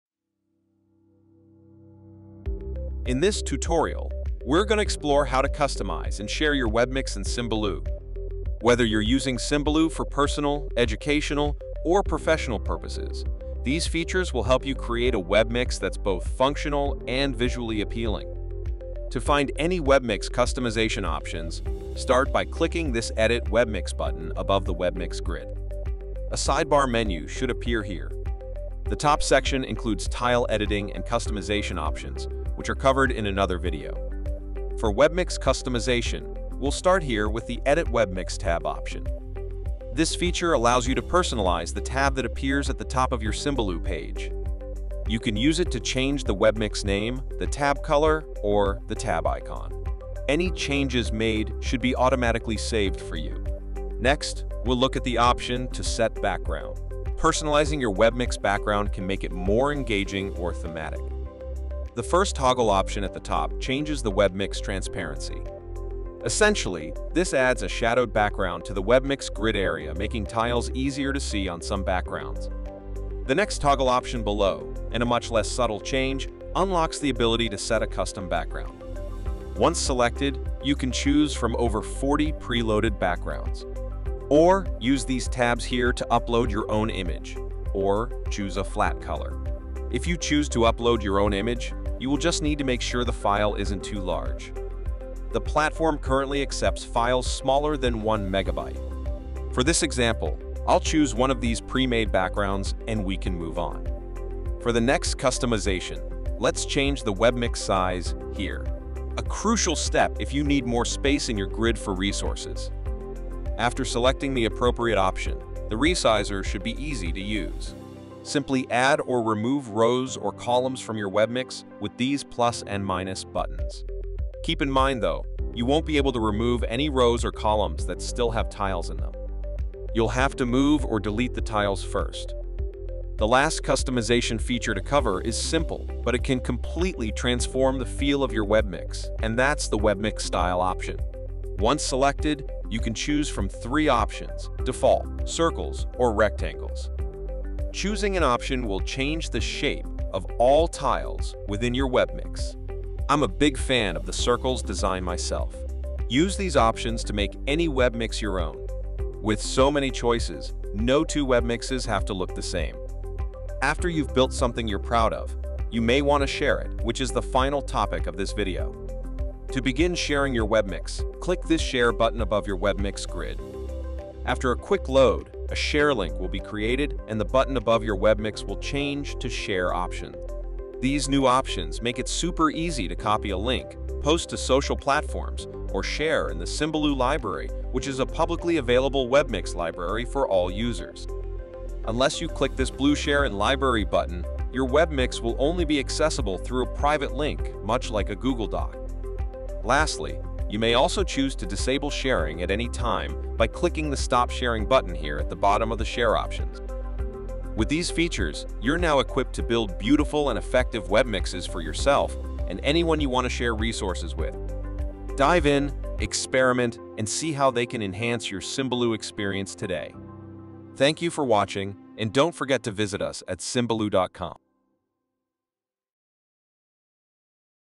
Webmix Customization & Sharing - Symbaloo in 2024
Create a beautiful, unique, and functional Symbaloo Webmix with these customization options. Learn how to edit your webmix tab, background, size, style, and more.

When you're ready, discover how to share your Webmix with others in just a few clicks!

Create a free account today at Symbaloo.com

0:00 - Editing a Webmix
0:35 - Webmix Tab
0:53 - Webmix Background
1:44 - Webmix Grid SIze
2:12 - Webmix Style
2:42 - Webmix Sharing

Video Transcription:

Hello! In this tutorial, we're going to explore how to customize and share your Webmix in Symbaloo.

Whether you're using Symbaloo for personal, educational, or professional purposes, these features will help you create a Webmix that’s both functional and visually appealing.

To find any Webmix customization options, start by clicking this 'Edit Webmix' button above the Webmix grid.

A sidebar menu should appear here. The top section includes Tile Editing and Customization options, which are covered in another video.

For Webmix Customization, we'll start here with the 'Edit Webmix Tab' option.

This feature allows you to personalize the tab that appears at the top of your Symbaloo page.

You can use it to change the Webmix name, the Tab color, or the Tab icon. Any changes made should be automatically saved for you.

Next, we'll look at the option to 'Set Background'. Personalizing your Webmix background can make it more engaging or thematic.

The first toggle option at the top changes the Webmix transparency.

Essentially, this adds a shadowed background to the Webmix grid area - making Tiles easier to see on some backgrounds.

The next toggle option below, and a much less subtle change, unlocks the ability to Set a Custom Background.

Once selected, you can choose from over 40 pre-loaded backgrounds. Or, use these tabs here to Upload your own image, or, choose a flat color.

If you choose to upload your own image, you will just need to make sure the file isn't too large. The platform currently accepts files smaller than 1 megabyte.

For this example, I'll choose one of these premade backgrounds and we can move on.

For the next customization, let's change the Webmix size, here. A crucial step if you need more space in your grid for resources.

After selecting the appropriate option, the resizer should be easy to use. Simply add or remove rows or columns from your Webmix with these plus and minus buttons.

Keep in mind though, you won't be able to remove any rows or columns that still have tiles in them. You'll have to move or delete the tiles first.

The last customization feature to cover is simple, but it can completely transform the feel of your Webmix - and that's the Webmix Style option.

Once selected, you can choose from three options: Default, Circles, or Rectangles.

Choosing an option will change the shape of all tiles within your Webmix. I'm a big fan of the Circles design myself.

Use these options to make any Webmix your own. With so many choices, no two Webmixes have to look the same.

After you've built something you're proud of, you may want to share it, which is the final topic of this video.

To begin sharing your Webmix, click this 'Share' button above your Webmix grid.

After a quick load, a share link will be created and the button above your Webmix will change to 'Share options'

These new options make it super easy to copy a link, post to social platforms, or share in the Symbaloo Library - which is a publicly available Webmix Library for all users.

Unless you click this blue 'Share in Library' button, your Webmix will only be accessible through a private link - much like a Google Doc.

Lastly, you may also choose to disable sharing at any time by clicking the 'Stop sharing' button here at the bottom of the share options.

With these features, you’re now equipped to build beautiful and effective Webmixes for yourself and anyone you want to share resources with!

Dive in, experiment, and see how they can enhance your Symbaloo experience today.